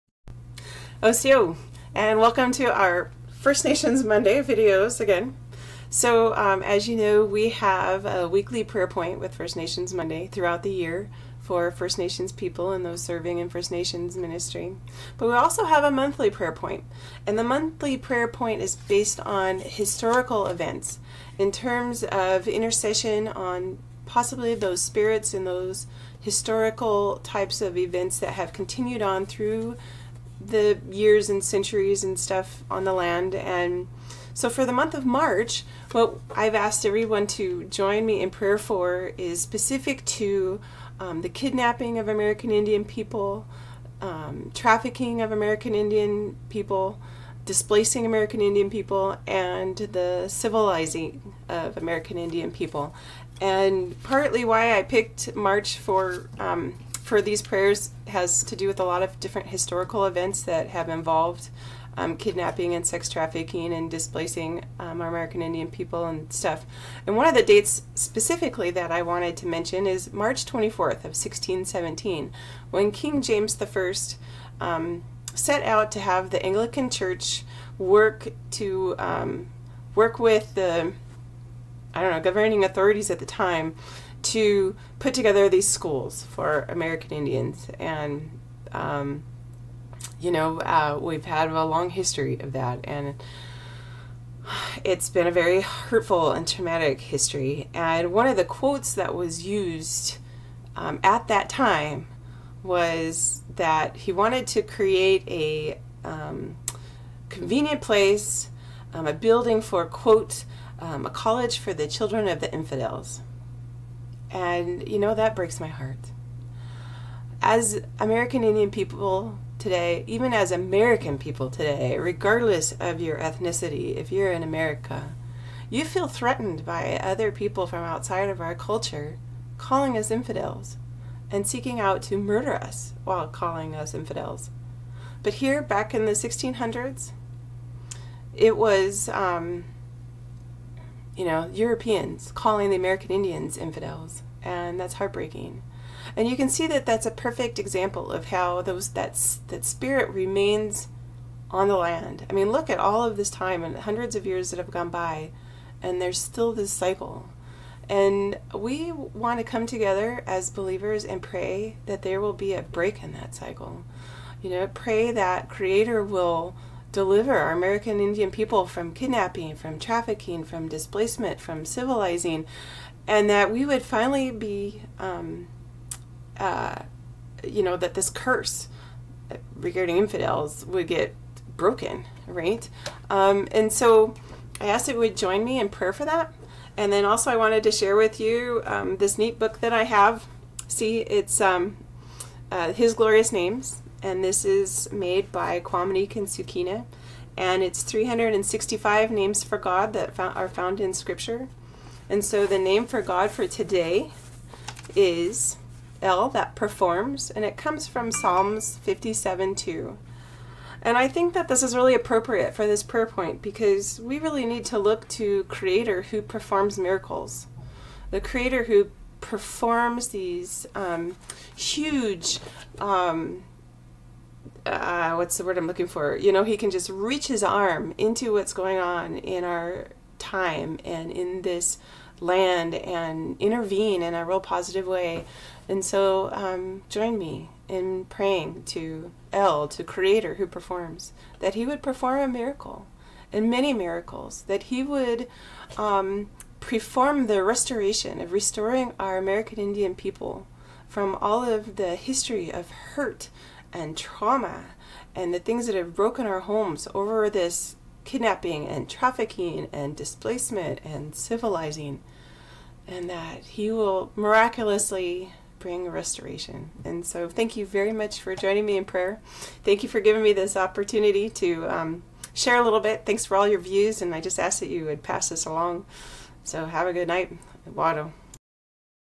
Kidnapping, Trafficking, Displacement and Civilizing of American Indian People
HISTORICAL PERSPECTIVE ON INTERCESSORY PRAYER FOR NATIVE AMERICA - Kidnapping, Trafficking, Displacement and Civilizing

Please join us in prayer for American Indian people and those serving in American Indian ministry regarding the history and spirit of kidnapping, sex trafficking, displacement and civilizing of American Indian people across Turtle Island.  Pray that "El that performs all things" Psalm 57:2 would perform miracles of restoration, healing and deliverance.

Special Thanks to Rev. Qaumaniq & Dr. Suuqiina for their book, "His Glorious Names: 365 names of YHWH with musical notes, modern and paleo Hebrew meanings"  This book is available at IndigenousMessengers.Com .